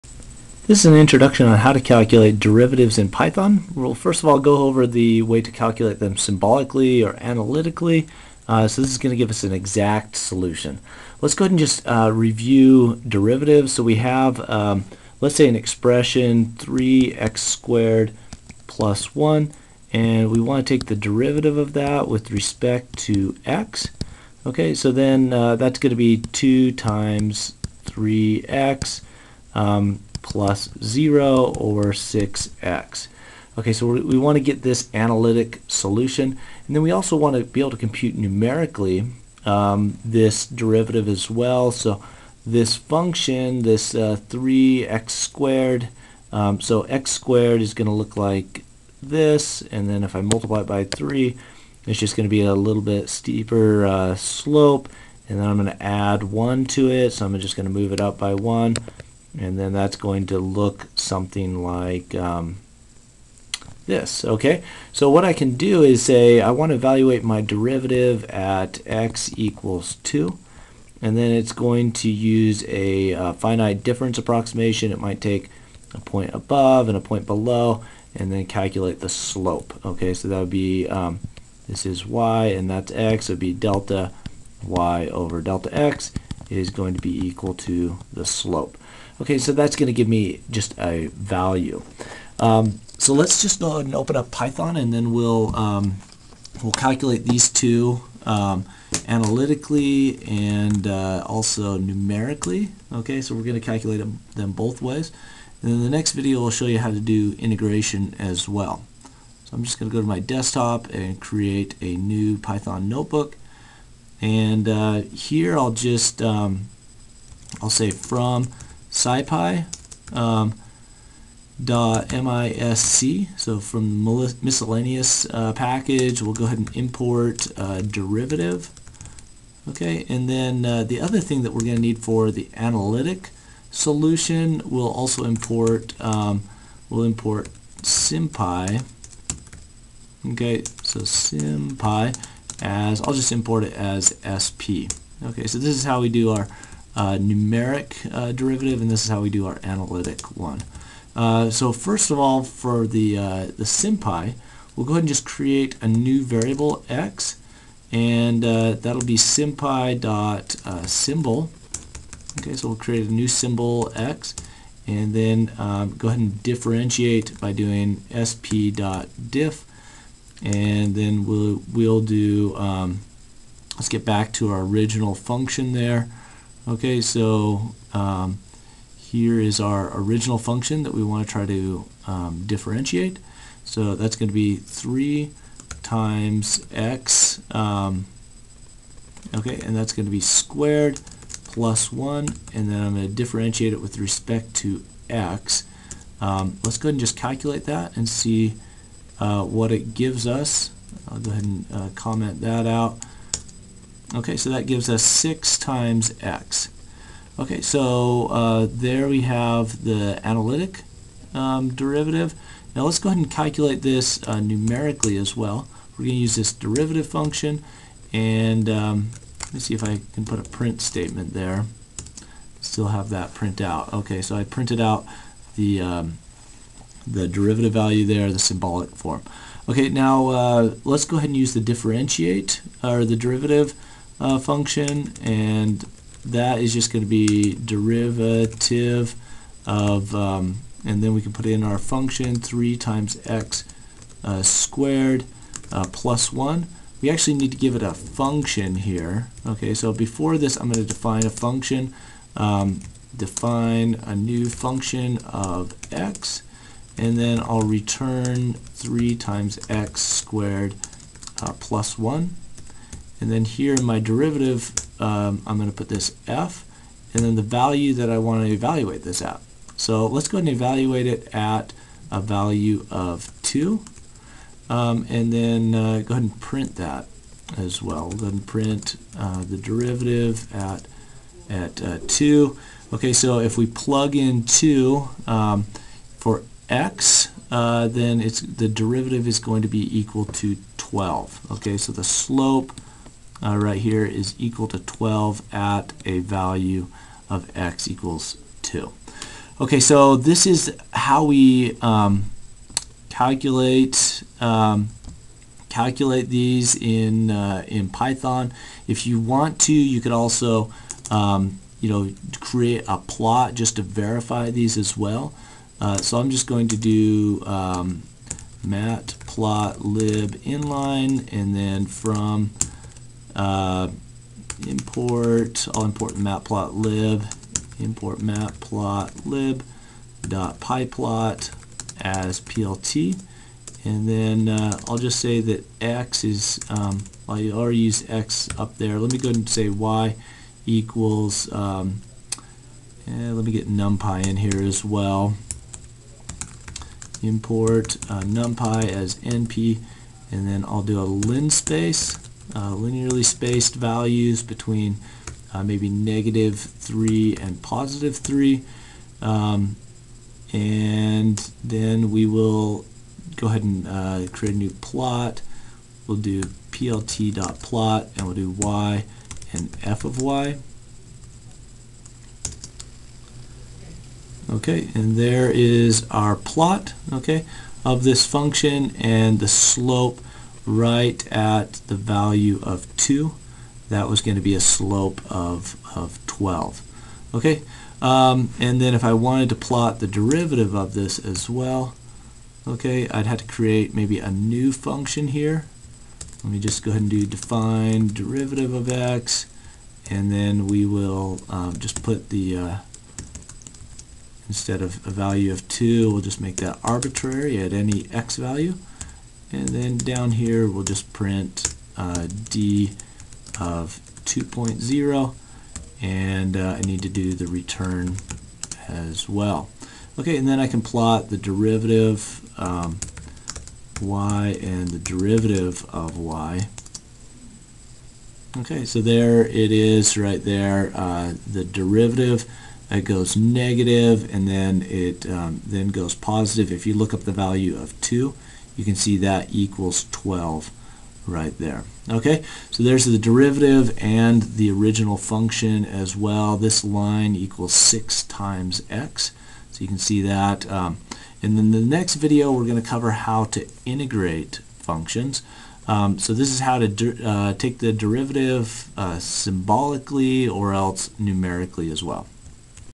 Derivatives, Symbolic and Numeric in Python
Exact analytical derivatives and numerical derivatives from finite differences are computed in Python with Sympy (Symbolic Python) and the Scipy.misc derivative function.